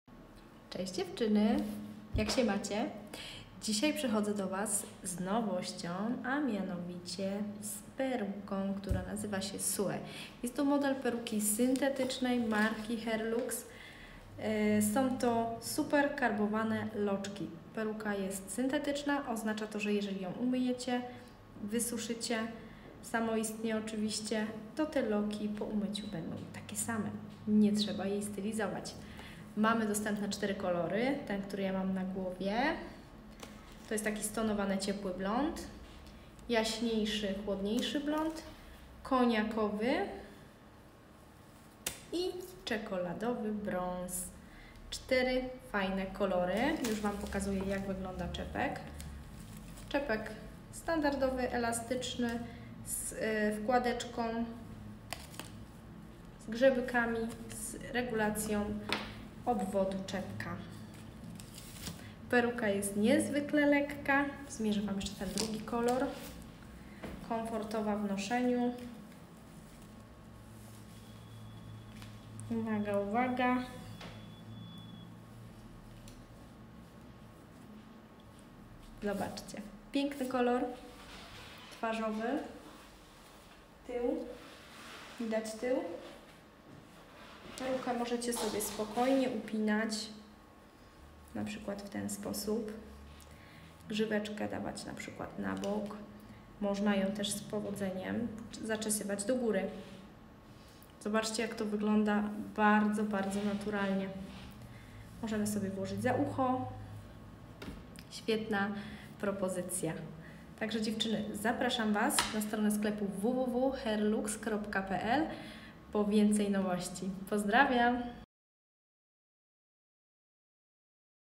Oryginalna peruka Sue marki Hair Lux!
Hej dziewczyny!
Przedstawiamy luksusową perukę syntetyczną firmy Hair Lux.

Modna fryzura, włosy falowane, pocieniowane. Peruka po umyciu i samoistnym wyschnięciu powraca do pierwotnego kształtu. Niezwykle lekka i komfortowa w noszeniu, czepek elastyczny przewiewny przód mono top. Taka konstrukcja zapewnia niezwykle naturalny wygląd przy czole, pozwala na upinanie włosów do góry.

Czepek – przód lace front bez konieczności obcinania.
Tył – regulacja obwodu.
Przedziałek – na środku.
Rozmiar – M 55-57 cm, z tyłu regulacja na rzepy.
Włos – termoochronny, temperatura prostowania lub kręcenia max. 120°C.

Pozdrawiamy 🥰